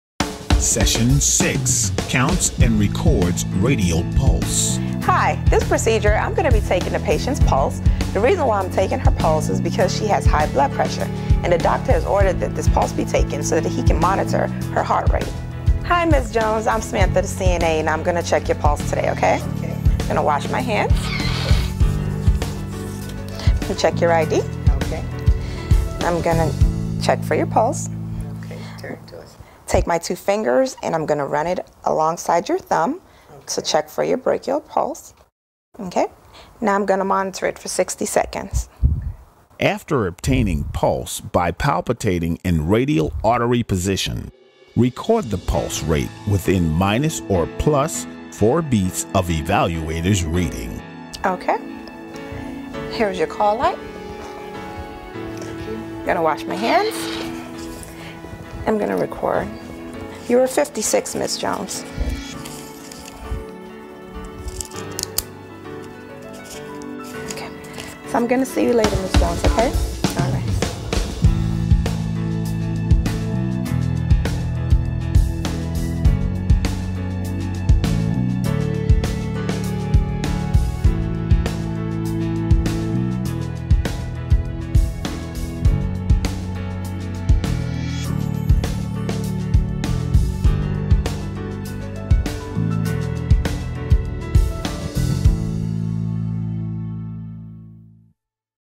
Session 6: Counts and Records Radial Pulse
In accordance with the Pearsons NNAAP (National Nurse Aide Assessment Program) Georgia Piedmont Technical College has taken the testing one step further to assist students and graduates in keeping focus on these skillsets. Please enjoy the brief tutorial and use this knowledge to prepare for the NNAAP Exam.